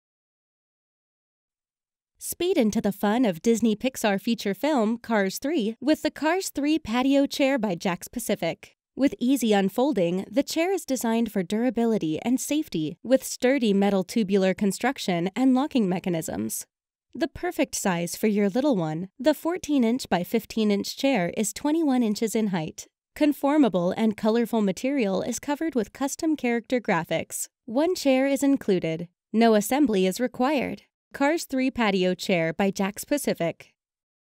Disney Cars 3 Patio Chair
Cars 3 Patio Chair- Item Number: 41882

Speed into the fun of Disney Pixar feature film Cars 3 with the Cars 3 patio chair by JAKKS Pacific. With easy unfolding the chair is designed for durability and safety with sturdy metal tubular construction and locking mechanisms. The perfect size for your little one, the 14 inch by 15 inch chair is 21 inches in height. Conformable and colorful material is covered with custom character graphics. One chair is included, no assembly is required. Cars 3 patio chair by JAKKS Pacific.